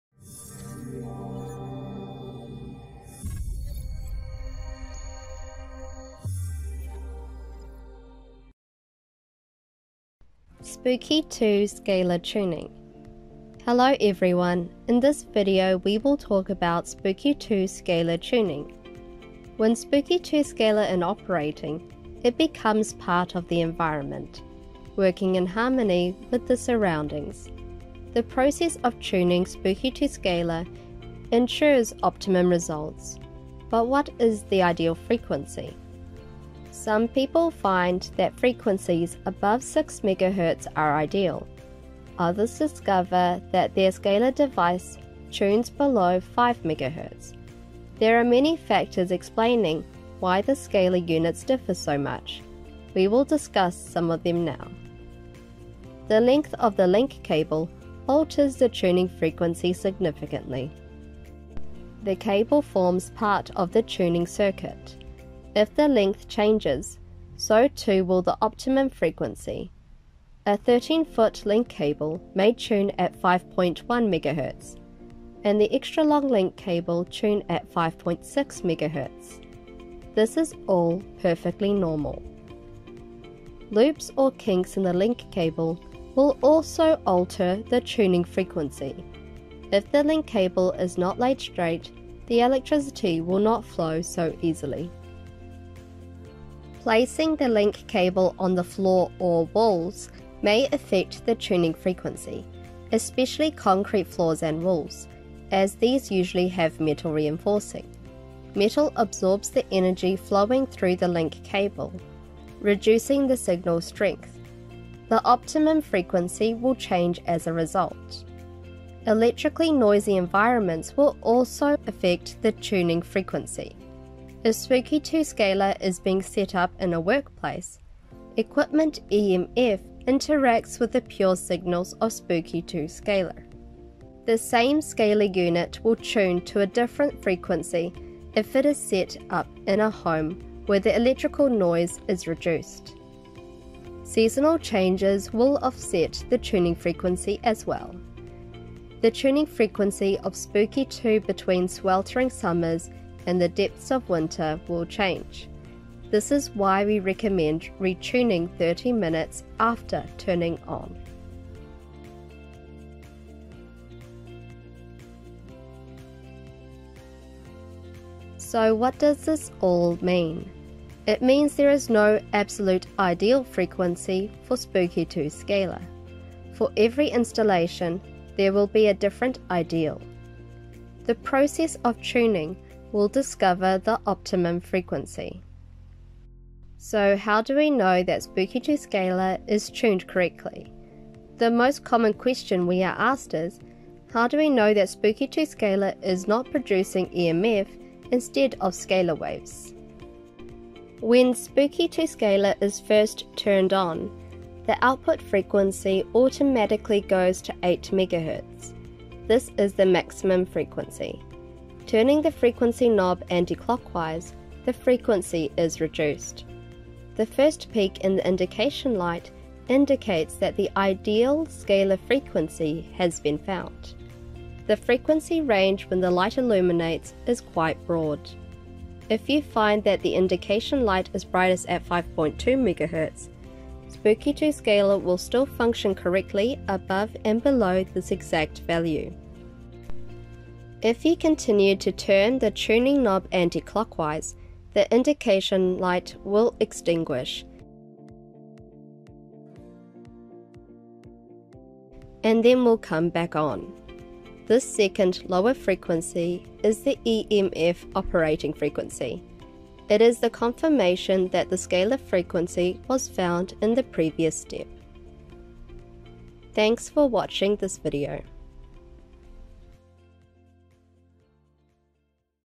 Spooky2 Scalar Tuning
When Spooky2 Scalar in operating, it becomes part of the environment, working in harmony with the surroundings. The process of tuning Spooky2 Scalar ensures optimum results. But what is the ideal frequency?

Some people find that frequencies above 6 MHz are ideal. Others discover that their scalar device tunes below 5 MHz. There are many factors explaining why the scalar units differ so much. We will discuss some of them now.

The length of the link cable alters the tuning frequency significantly. The cable forms part of the tuning circuit. If the length changes, so too will the optimum frequency. A 13-foot link cable may tune at 5.1 MHz, and the extra-long link cable tune at 5.6 MHz. This is all perfectly normal.

Loops or kinks in the link cable will also alter the tuning frequency. If the link cable is not laid straight, the electricity will not flow so easily.

Placing the link cable on the floor or walls may affect the tuning frequency. Especially concrete floors and walls, as these usually have metal reinforcing. Metal absorbs the energy flowing through the link cable, reducing the signal strength. The optimum frequency will change as a result.

Electrically noisy environments will also affect the tuning frequency. If Spooky2 Scalar is being set up in a workplace, equipment EMF interacts with the pure signals of Spooky2 Scalar. The same scalar unit will tune to a different frequency if it is set up in a home where the electrical noise is reduced.

Seasonal changes will offset the tuning frequency as well. The tuning frequency of Spooky2 between sweltering summers and the depths of winter will change. This is why we recommend retuning 30 minutes after turning on.

So what does this all mean? It means that there is no absolute “ideal” frequency for Spooky2 Scalar. For every installation, there will be a different “ideal”. The process of tuning will discover the optimum frequency.

So how do we know that Spooky2 Scalar is tuned correctly?
The most common question we are asked is “How do we know that Spooky2 Scalar is not producing EMF instead of Scalar waves?”

When Spooky2 Scalar is first turned on, the output frequency automatically goes to 8 MHz. This is the maximum frequency. Turning the frequency knob anticlockwise, the frequency is reduced. The first peak in the indication light indicates that the “ideal” scalar frequency has been found. The frequency range when the light illuminates is quite broad. If you find that the indication light is brightest at 5.2 MHz, Spooky2 Scalar will still function correctly above and below this exact value.

If you continue to turn the tuning knob anticlockwise, the indication light will extinguish, and then will come back on. This second, lower frequency is the EMF operating frequency. It is the confirmation that the scalar frequency was found in the previous step.